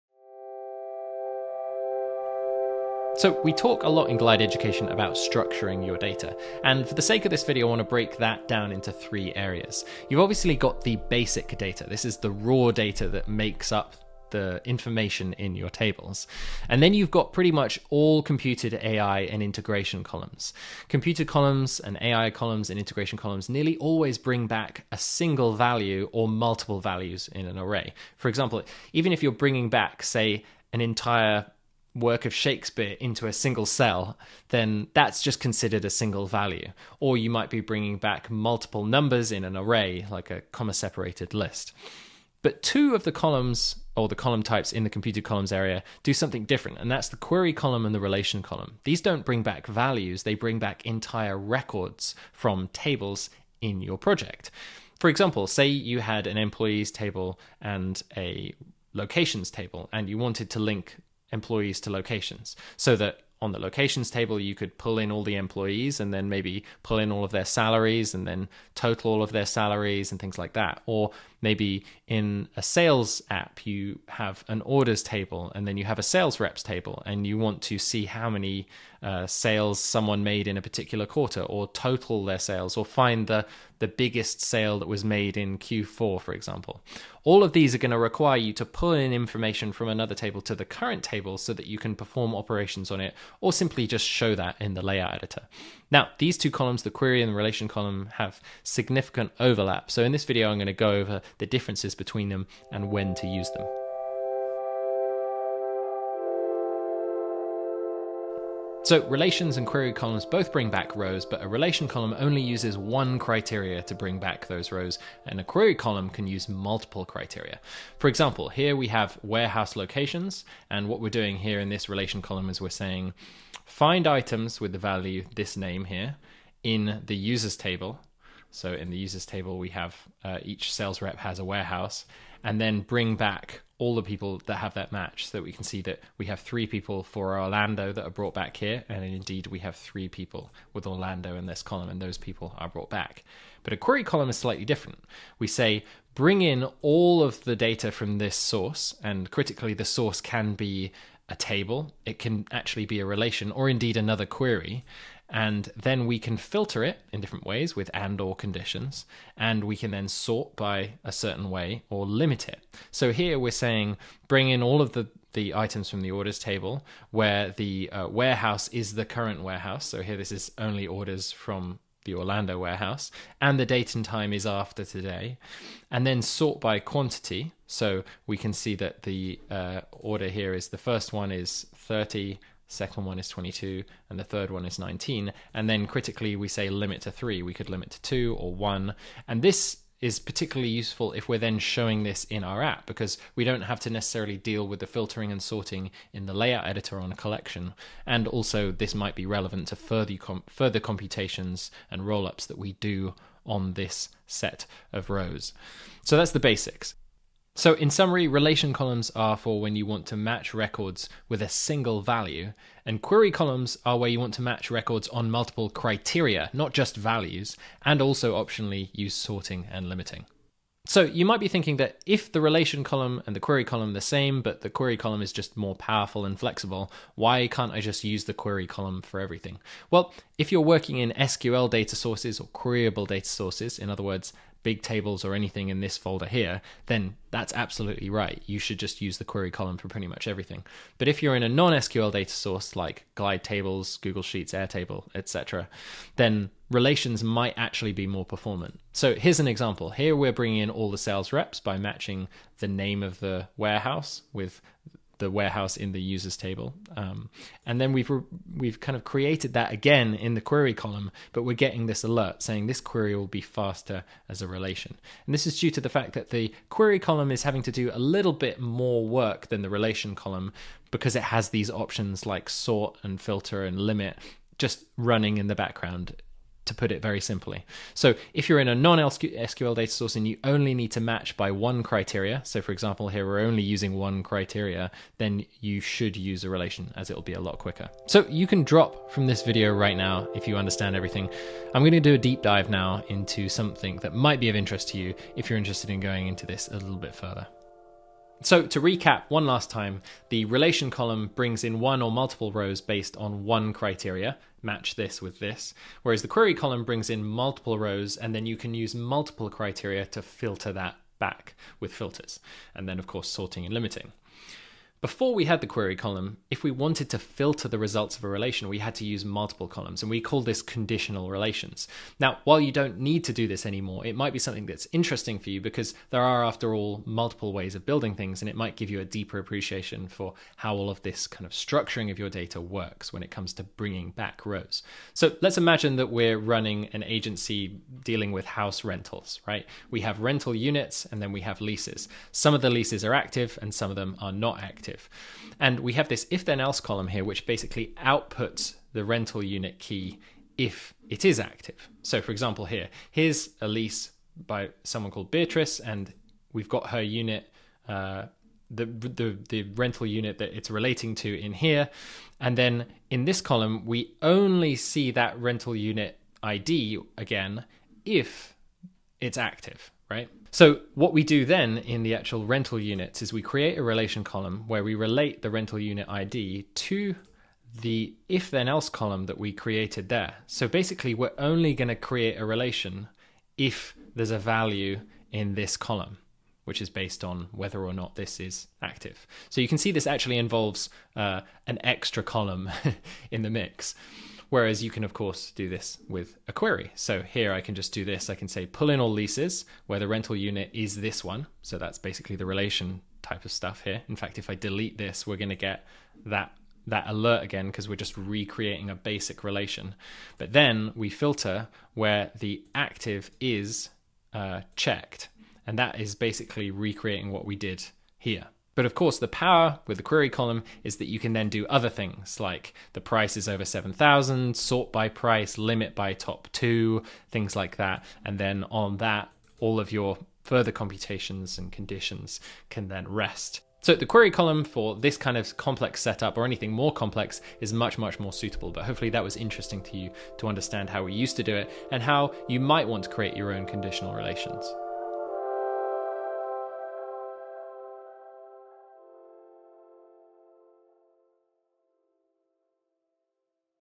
Relations vs Query Columns | Glide Apps Tutorial | No Code
Are you struggling with organizing and structuring your data in Glide? In this video, we dive into the importance of data organization and highlight the key areas to focus on: basic data, computed columns, integration columns, query columns, and relation columns.

When it comes to computed columns and integration columns, they often bring back single or multiple values, helping you streamline your data analysis. On the other hand, query columns and relation columns play a crucial role in bringing back rows of data, enhancing your data retrieval process.

Query columns offer the flexibility of matching records based on multiple criteria, enabling you to find specific information efficiently. Conversely, relation columns are designed to use a single criterion to bring back relevant rows of data, simplifying your data structuring process.

By understanding the importance of these different data areas and columns, you can unlock the true potential of Glide and optimize your data organization strategy. Whether you're a beginner or an experienced Glide user, this video will provide you with valuable insights and tips to enhance your data management skills.